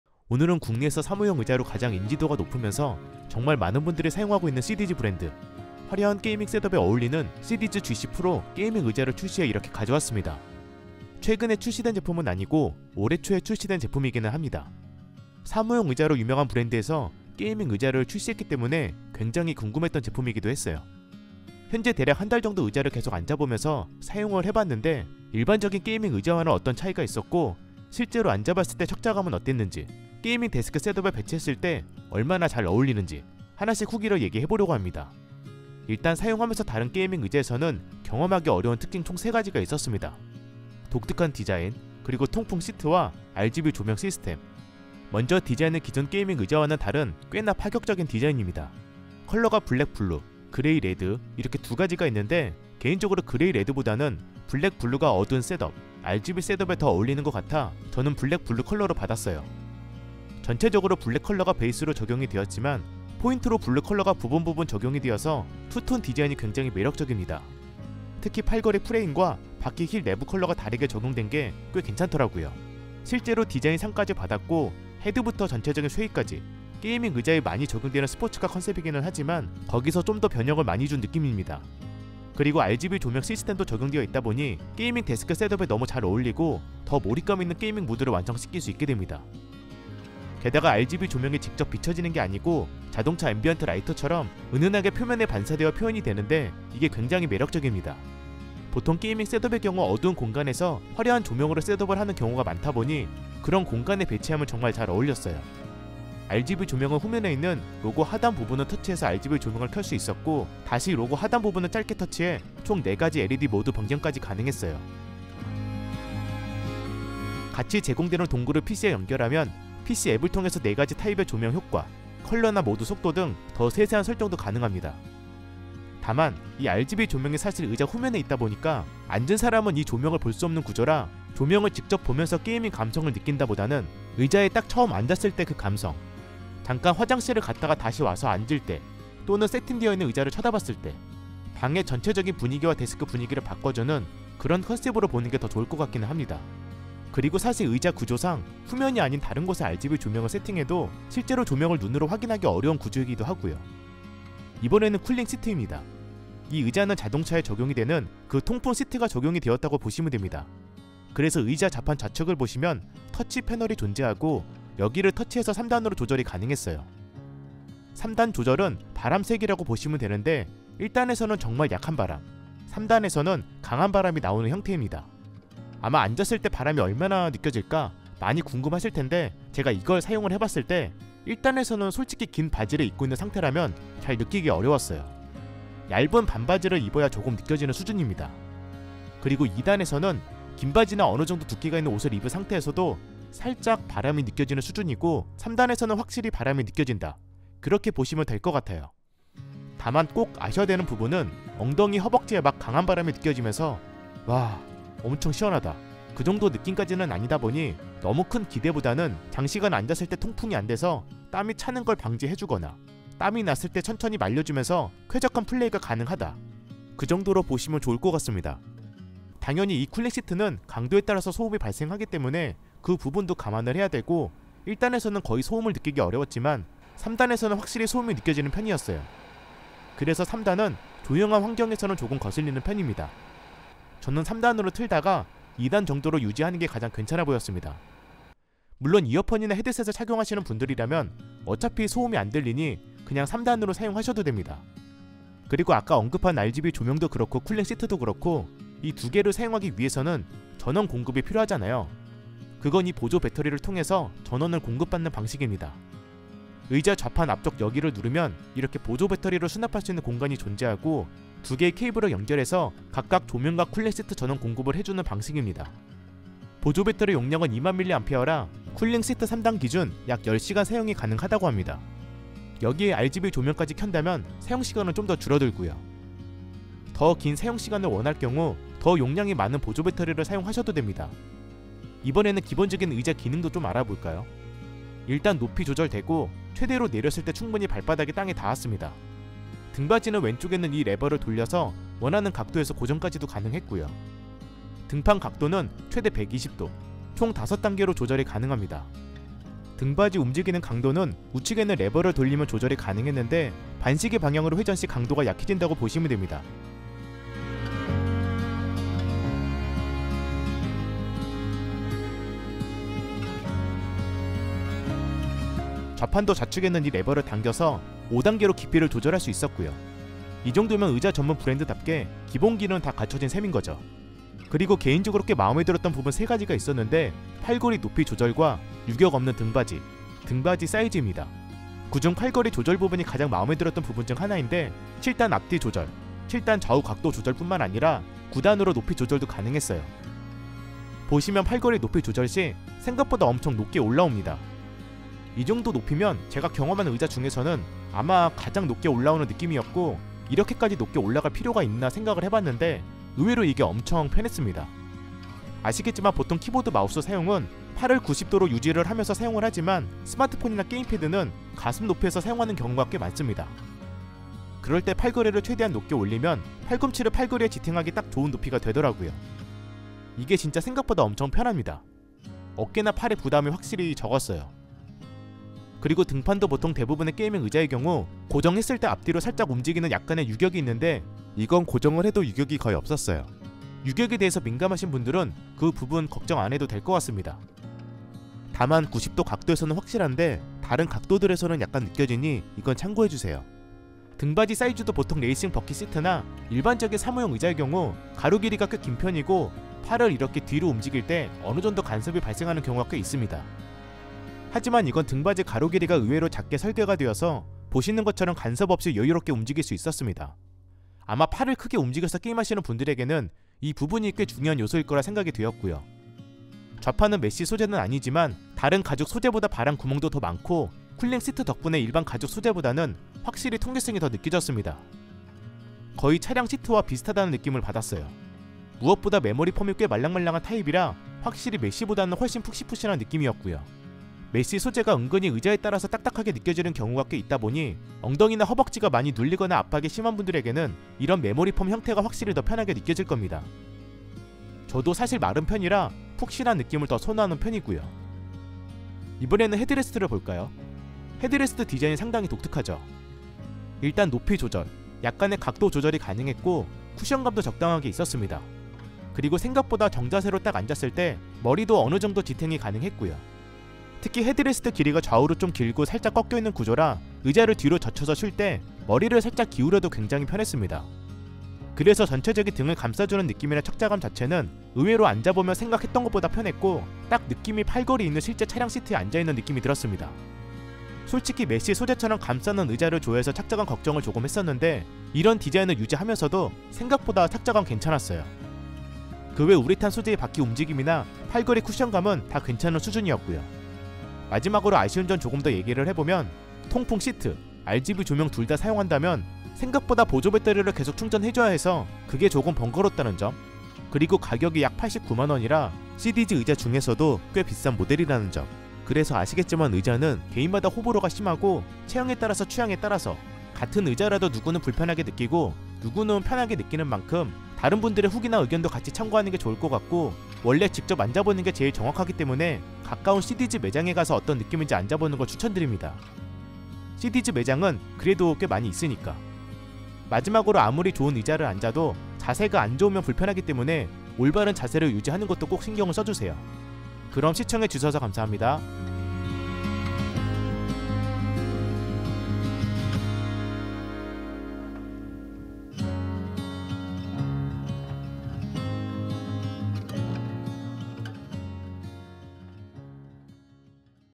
화려한 게이밍 데스크 셋업에 가장 잘 어울리는 의자! RGB 조명과 통풍시트가 탑재된 시디즈 GC PRO 게이밍 의자!
* 시디즈로부터 제품과 제작비를 지원받아 제작되었습니다

✔ Instagram - 일상을 보실 수 있습니다!